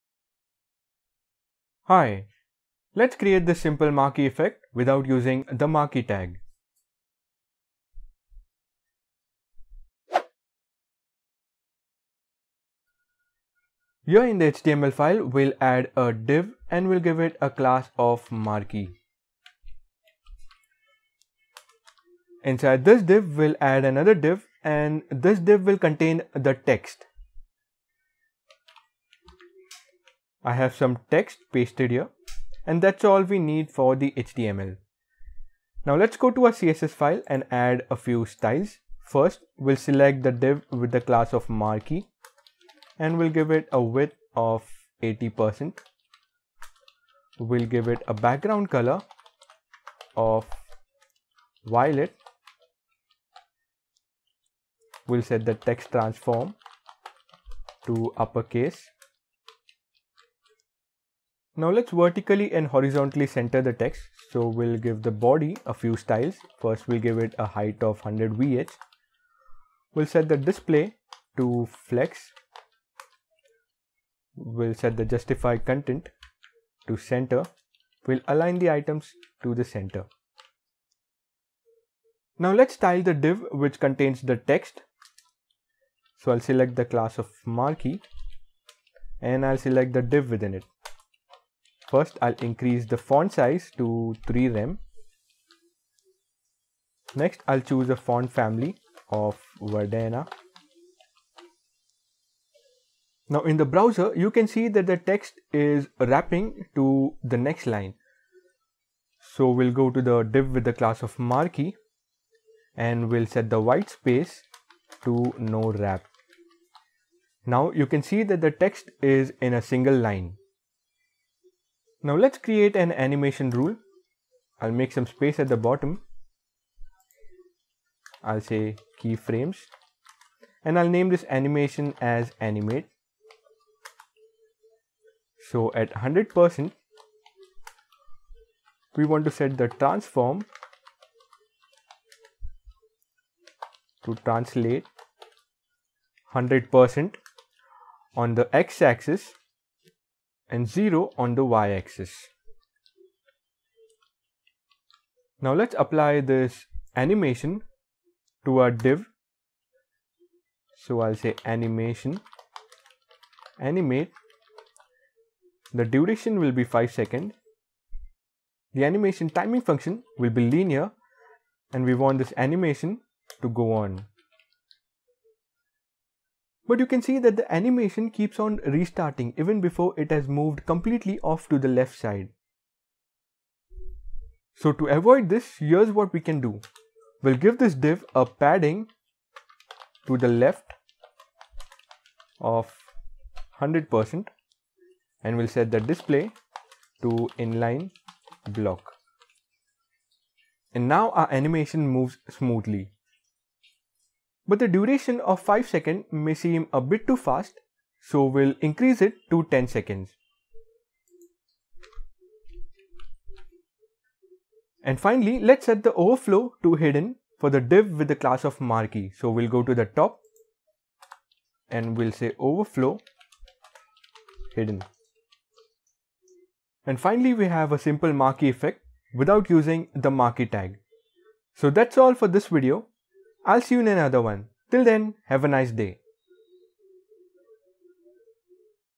Marquee Effect in HTML and CSS
In this video, let's try creating a marquee text effect without using the HTML marquee tag.